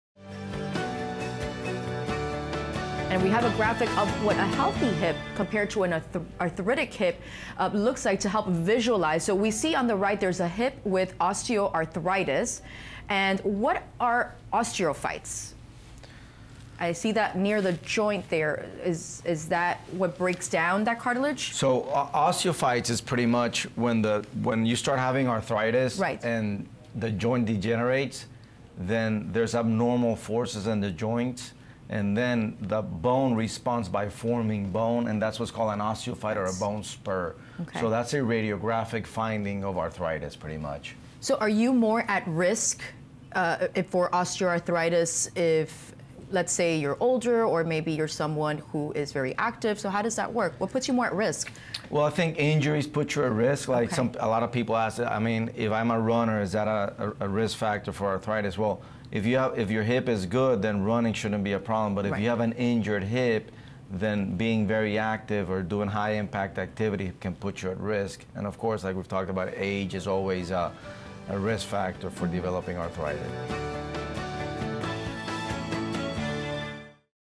Osteoarthritic Hip vs. Healthy Hip
What are osteophytes? What are the differences between a healthy hip and an osteoarthritic hip? Dr. Juan Carlos Suarez, Orthopedic Surgeon with Miami Orthopedics and Sports Medicine Institute, explains the degeneration of the joints, and how the bone responds.